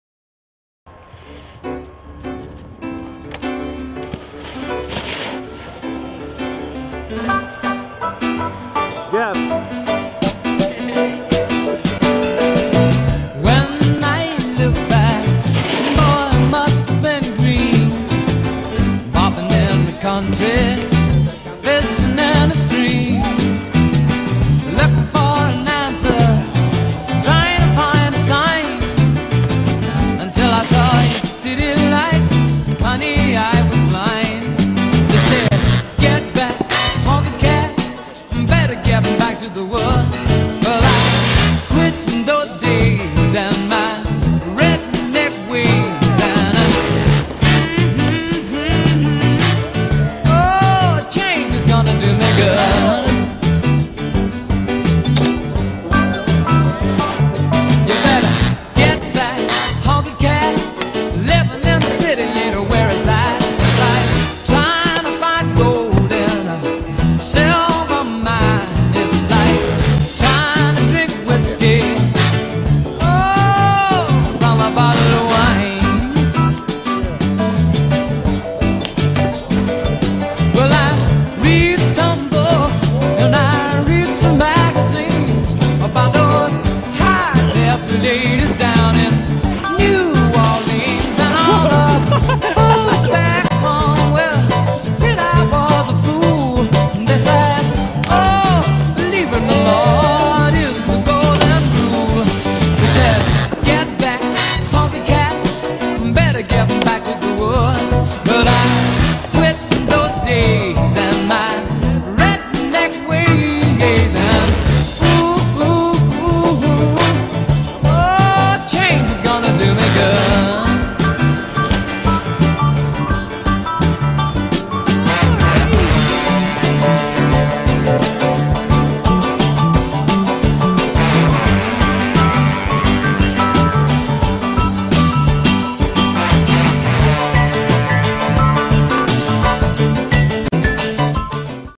ASHBURY DEMO 2013
Riders in order of appearance: Jordan Small, Ben Bilocq, Austin Leonard, Cameron Strand, Jesse Paul, Chris Bradshaw, Cole Linzmeyer, Johnny Miller, Josh Parker, Justin Fronius, Cody Beiersdorf, Justin Mulford, and Jake Kuzyk.

Shot and edited by Ransacker.